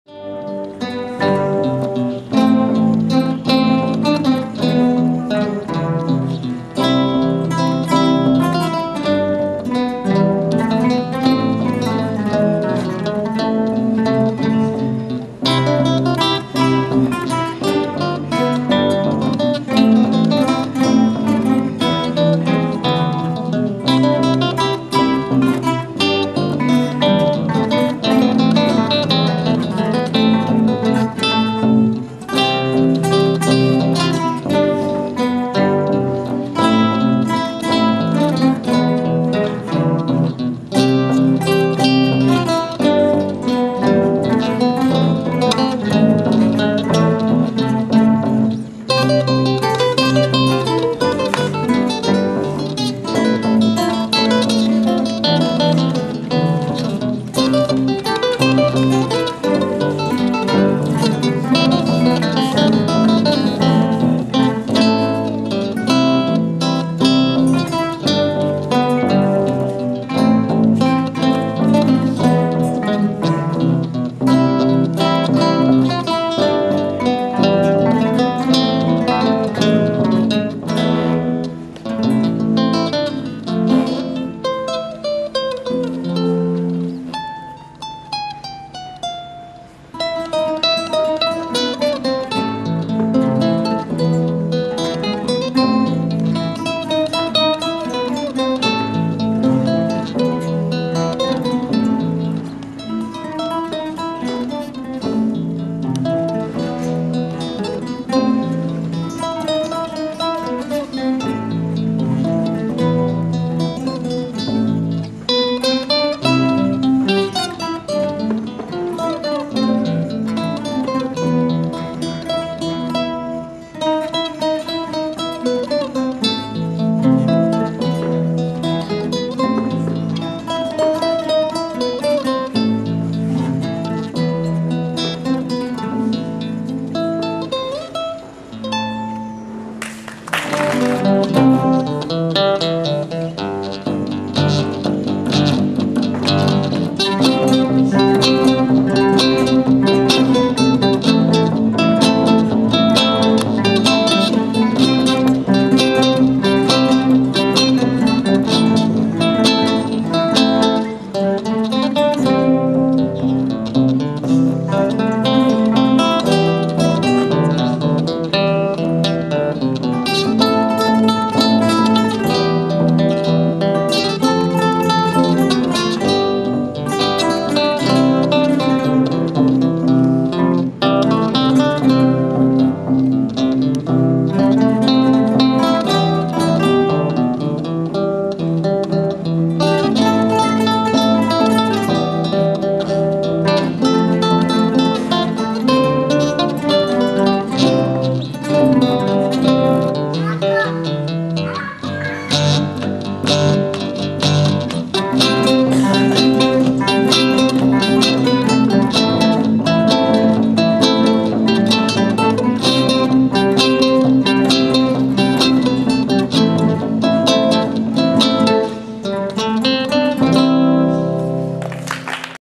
ELS GUITARRISTES ROIG TESALIA 2010 PART 4
Concert / classe oberta de guitarres, alumnes de Pere i Alba Salicrú